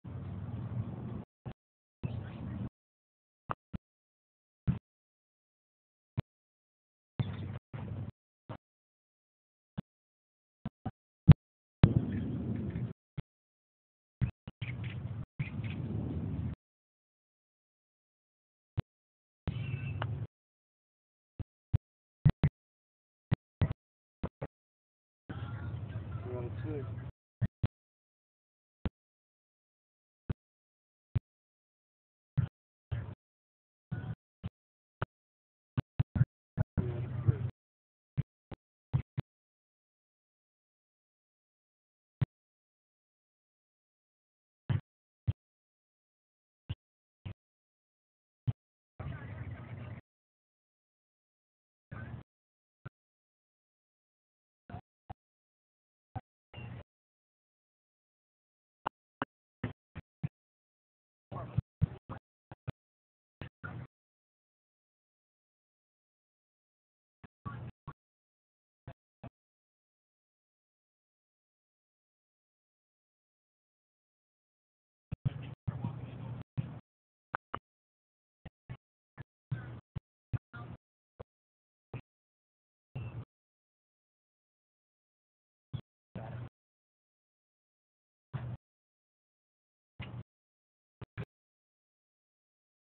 Blindstrikegolf
PGA TOUR player Steve Alker getting in some serious practice...always with blindstrike at the ready